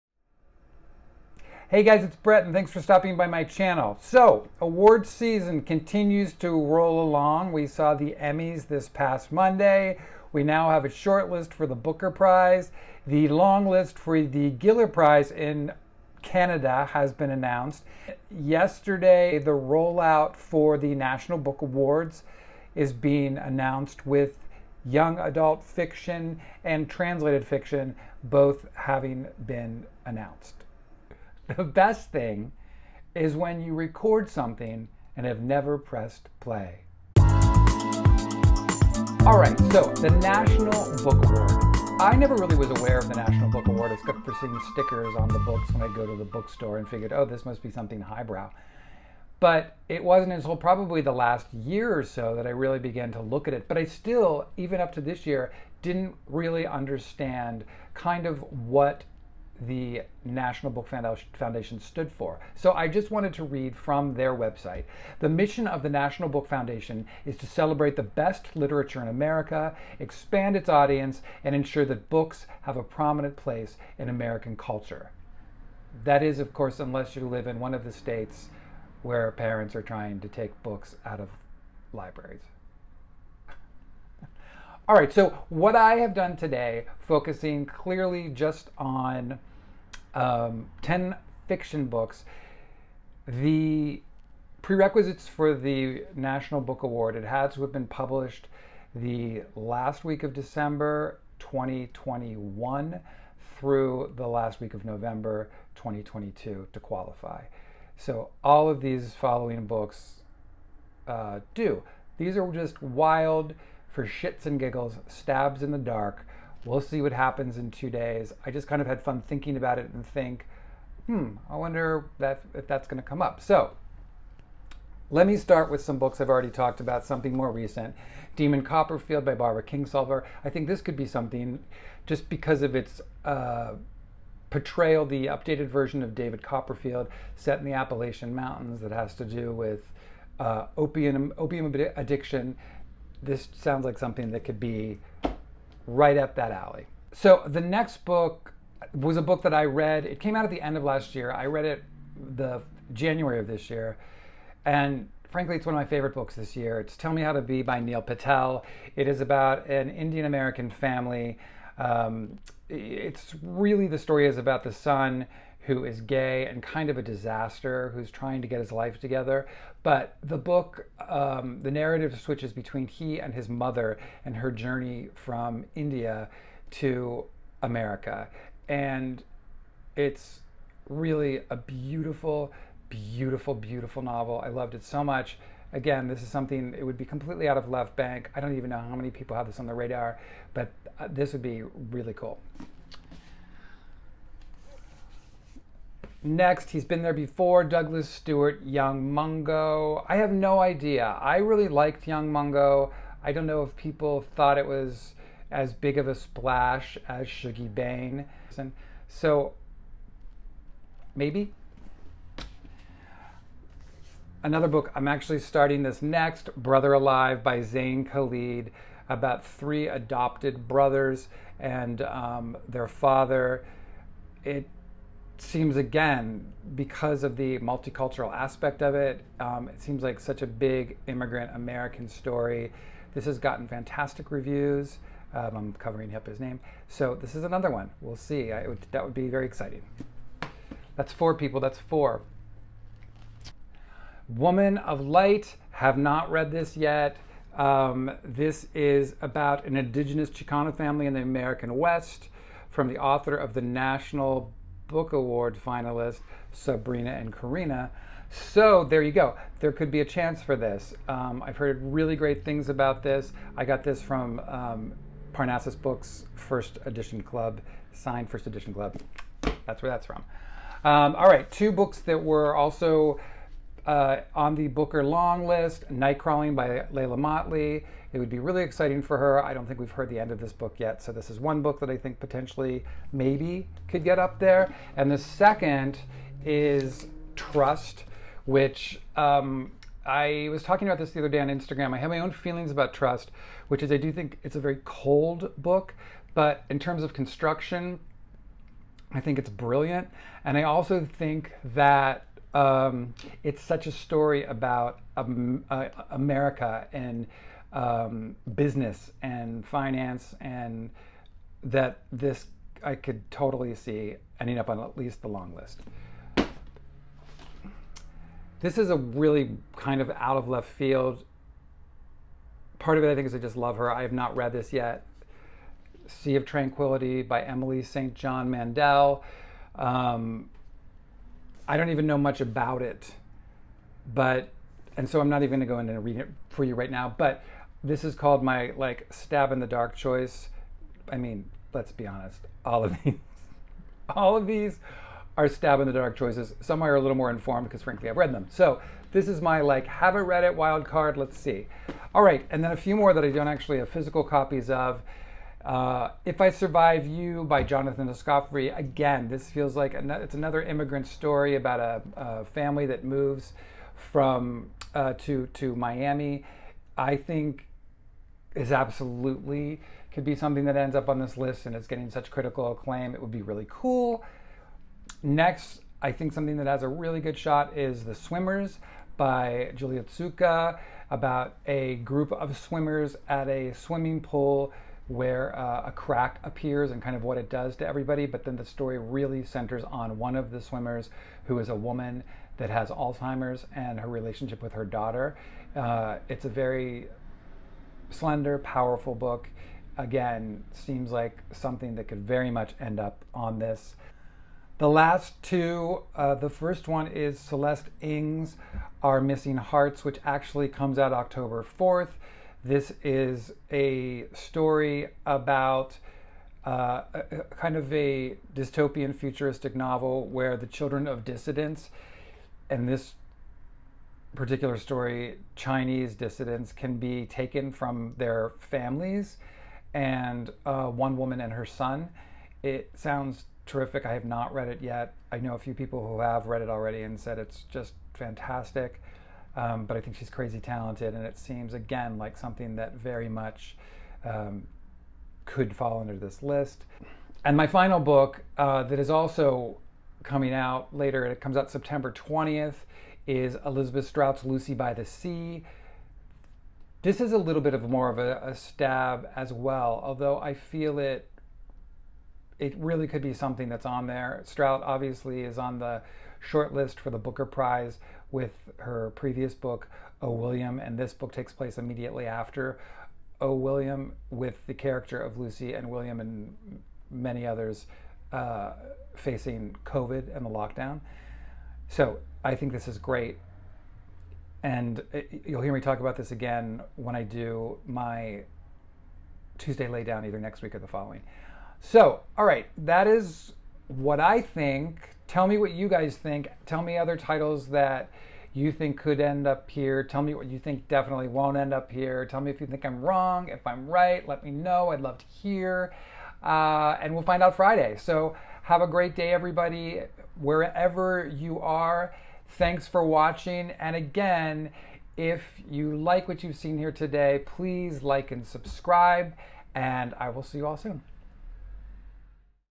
National Book Award Fiction Longlist Guesses
Taking some wild stabs in the dark I talk about ten books I think could make the NBA longlist which is announced Friday the 16th.
Come see if you agree or think I'm way off base!!!